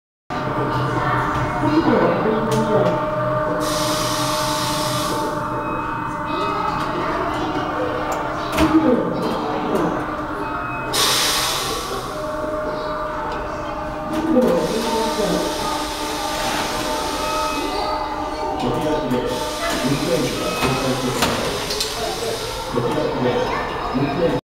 運転士シミュレータ・交通科学博物館・・Ⅰ
大阪市港区の交通科学博物館で運転士シミュレータを見ました。